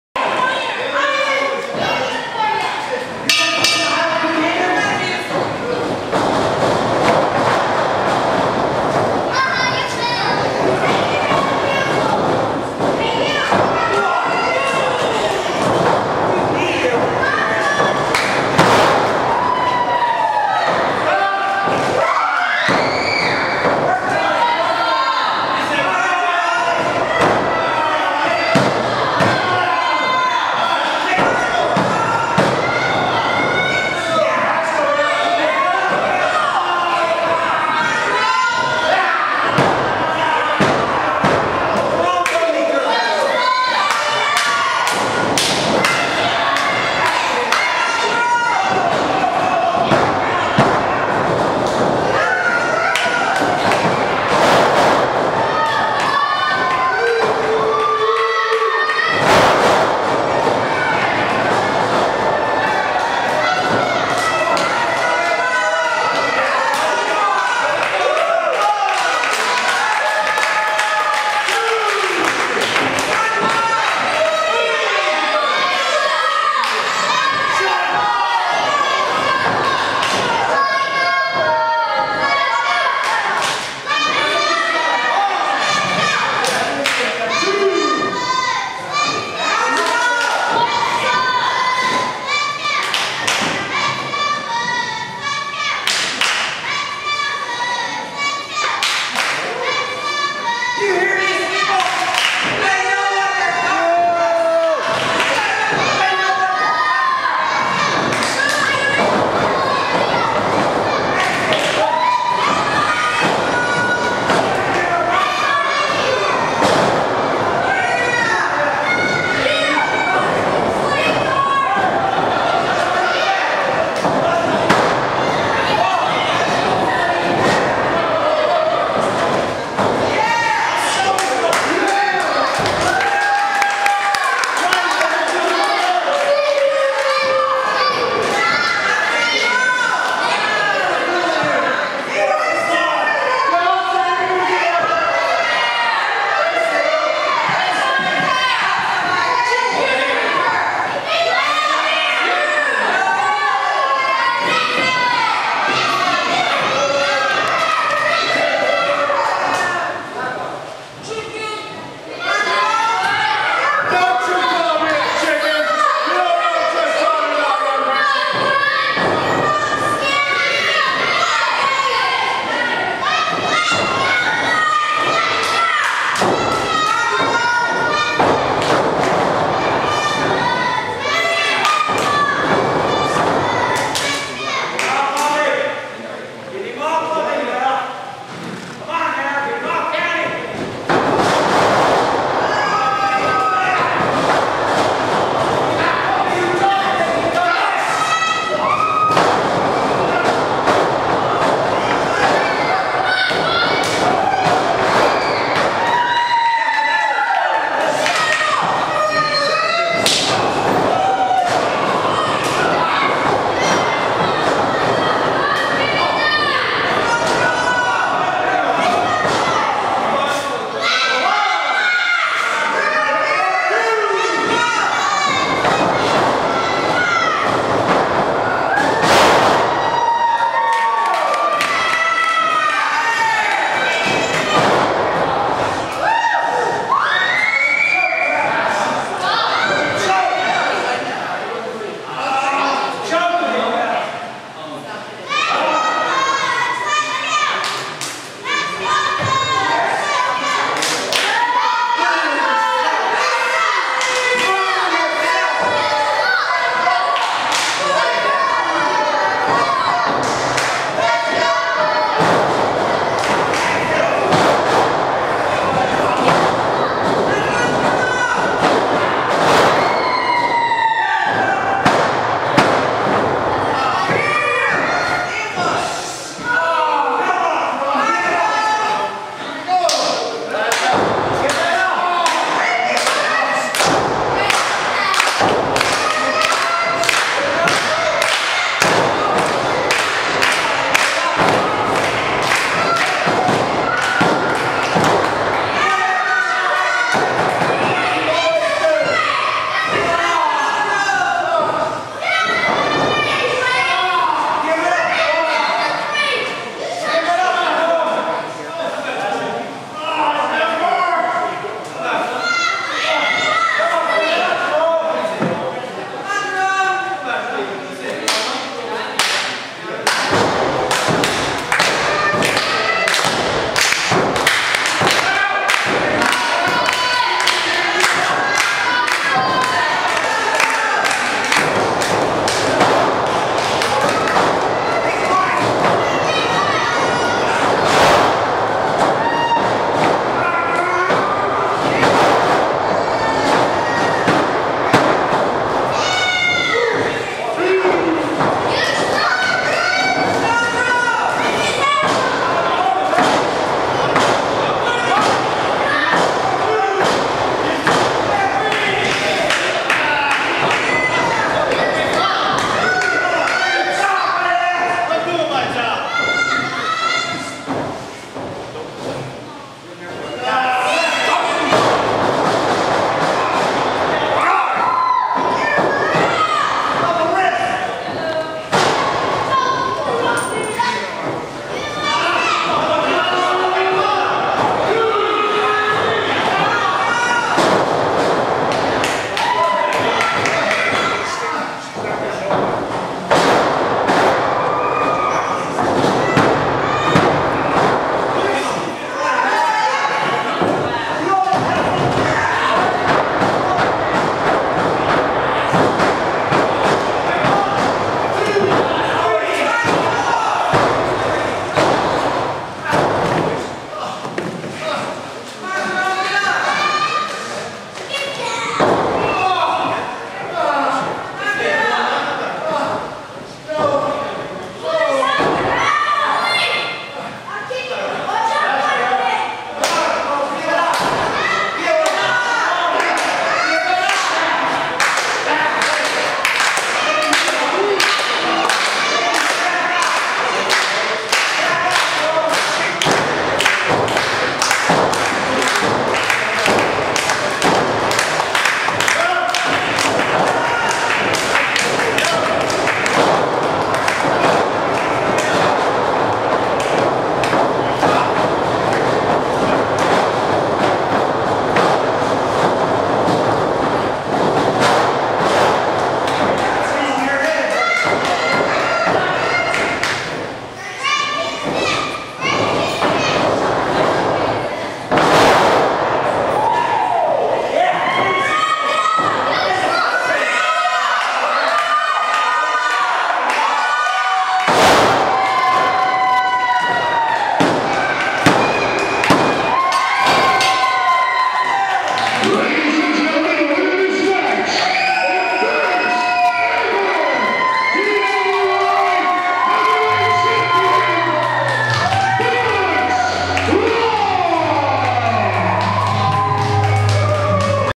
DWI WRESTLING'S First ever championship match Butch Long VS Jerome Hendrix June13,2010Movie_0001.wmv
DWI WRESTLING'S First ever championship match Butch Long VS Jerome Hendrix June13,2010